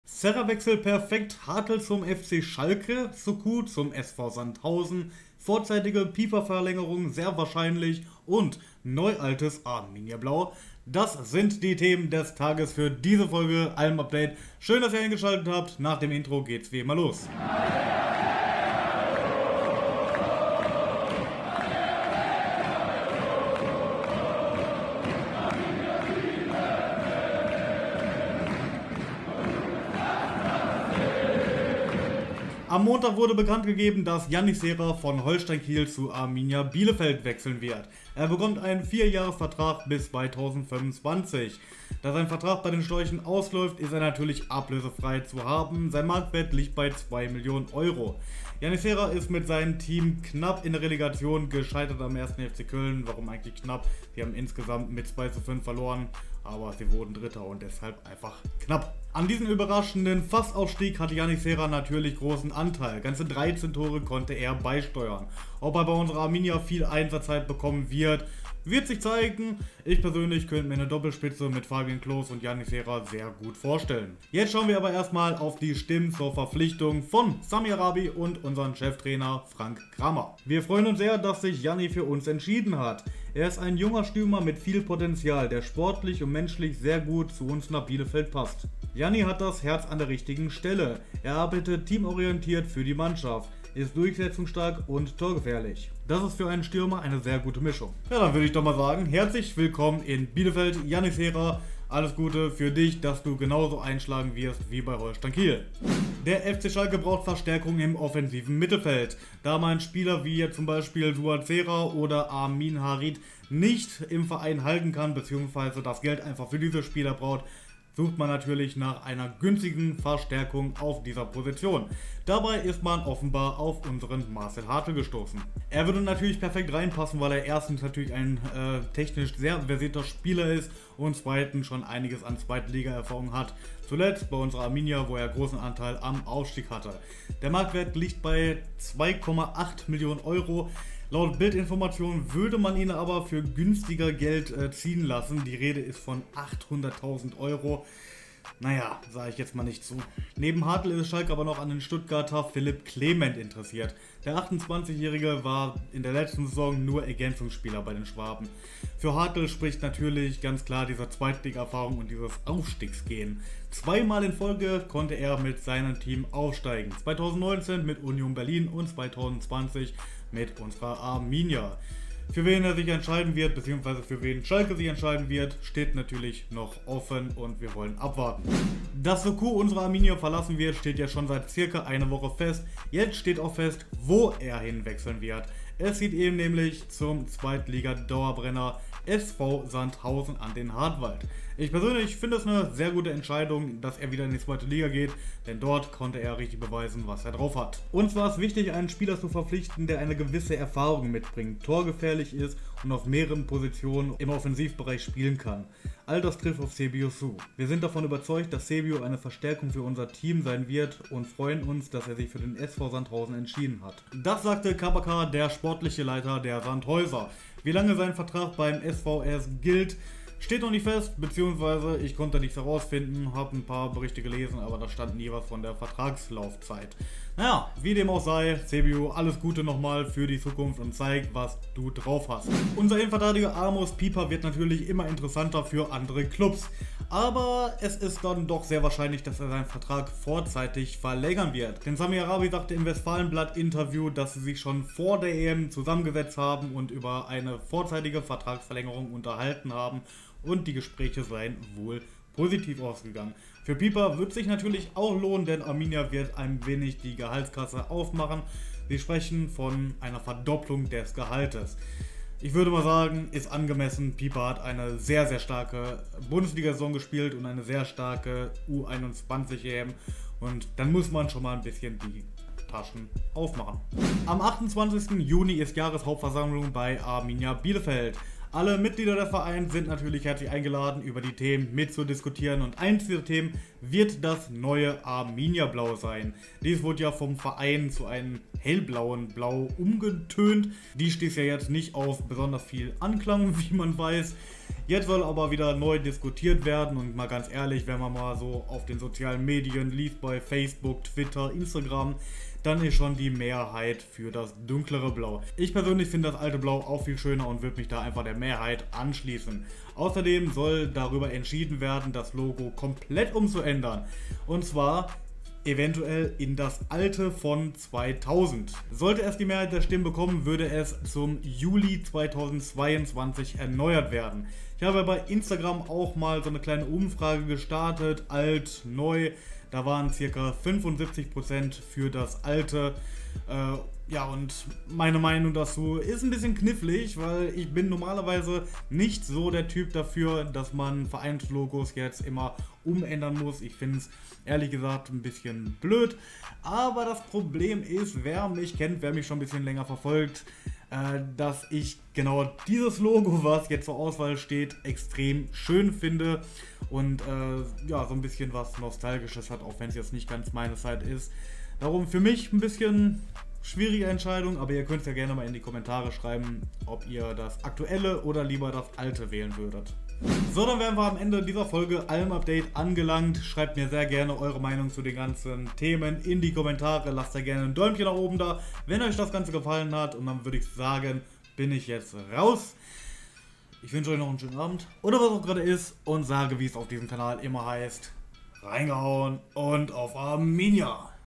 Alm-Update #13 - Serra-Wechsel perfekt, Hartel nach Schalke?, Soukou zum SV Sandhausen und mehr..
Neue Folge Alm-Update.

Das sind die heutigen Themen:

- Serra Wechsel perekt

- Hartel zum FC Schalke ?

- Soukou zum SV Sandhausen!

- Vorzeitige Pieper Verlängerung sehr wahrscheinlich

- Neualtes Arminiablau?